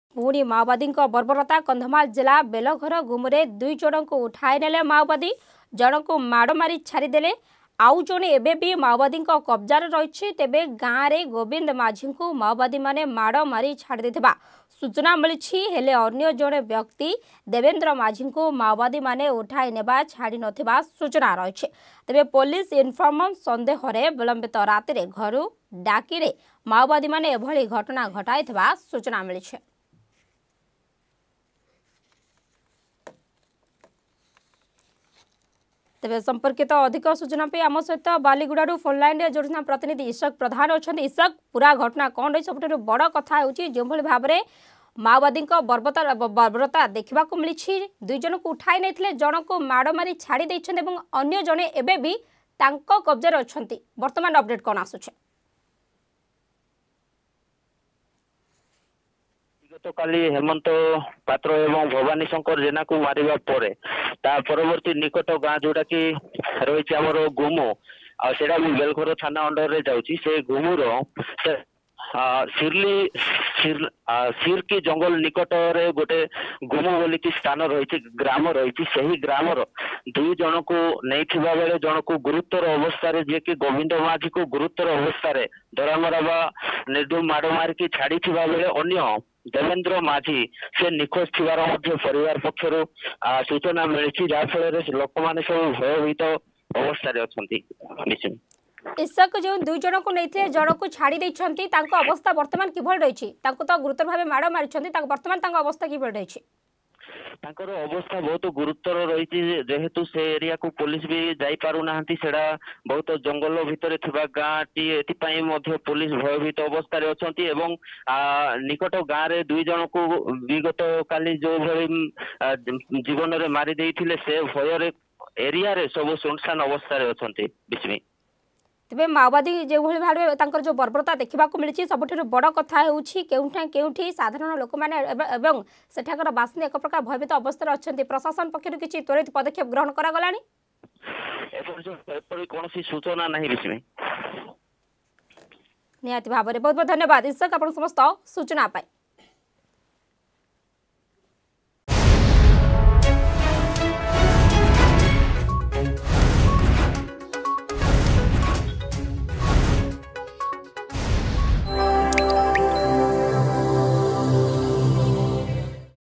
ପୁଣି ମାଓବାଦୀଙ୍କ ବର୍ବରତା
କନ୍ଧମାଳ ଜିଲ୍ଲା ବେଲଘର ଗୁମୁରେ ଦୁଇ ଜଣଙ୍କୁ ଉଠାଇନେଲେ ମାଓବାଦୀ । ଜଣଙ୍କୁ ମାଡ଼ ମାରି ଛାଡିଦେଲେ... ଆଉ ଜଣେ ଏବେ ବି ମାଓବାଦୀଙ୍କ କବଜାରେ.
ଅଧିକ ଖବର ପାଇଁ ଡ଼ାଉନଲୋଡ କରନ୍ତୁ “Argus News” ଆପ୍